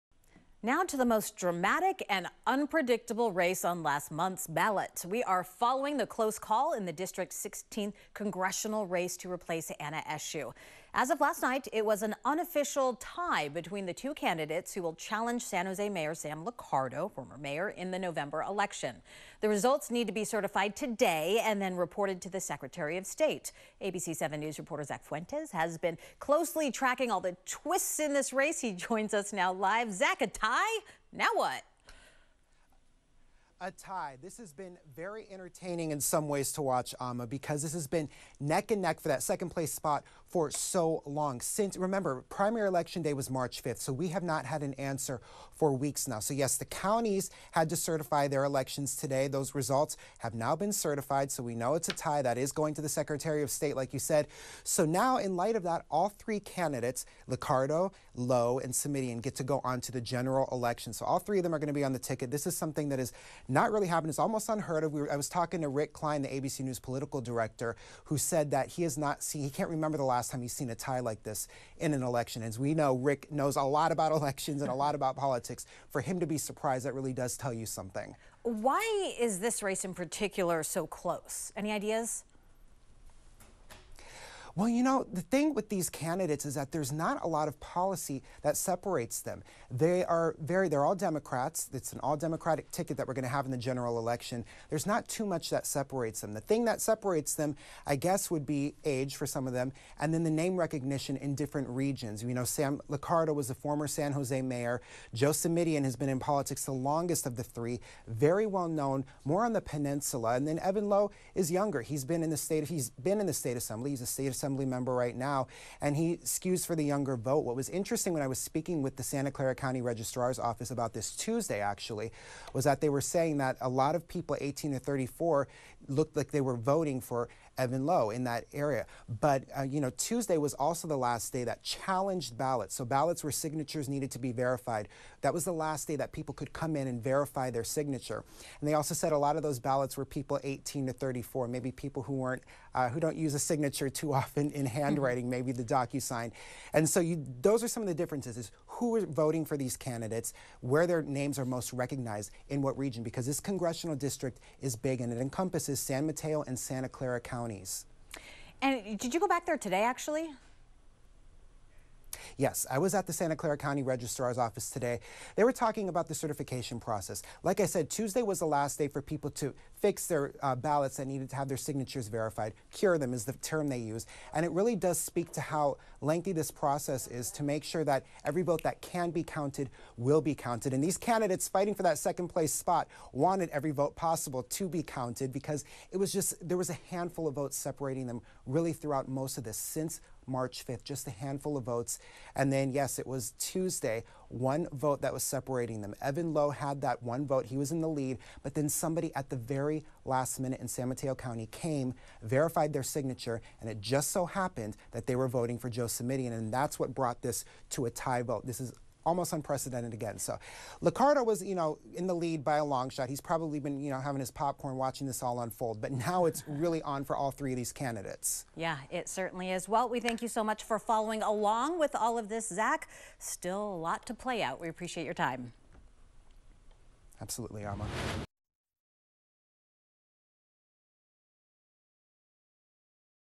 Rare 2nd place tie moves both Joe Simitian, Evan Low forward in CA District 16 Congressional race
A rare second place tie moves both San Mateo County Supervisor Joe Simitian and California Assemblymember Evan Low forward in the California District 16 Congressional race.